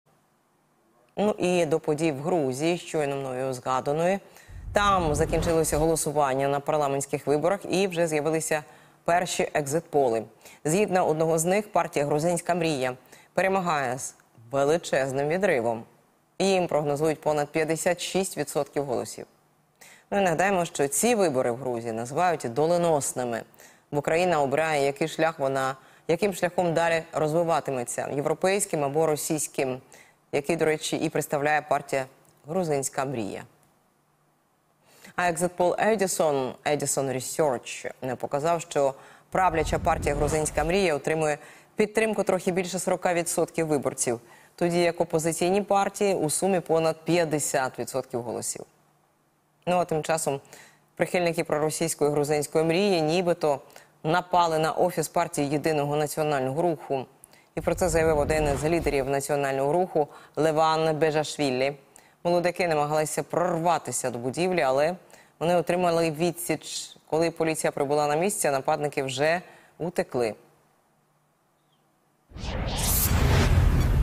😱 ЩОЙНО! Екзитполи оприлюднили РЕЗУЛЬТАТИ парламентських виборах у Грузії!
За даними екзитполу компанії Edison Research, опозиційні сили підтримали 51,9% виборців.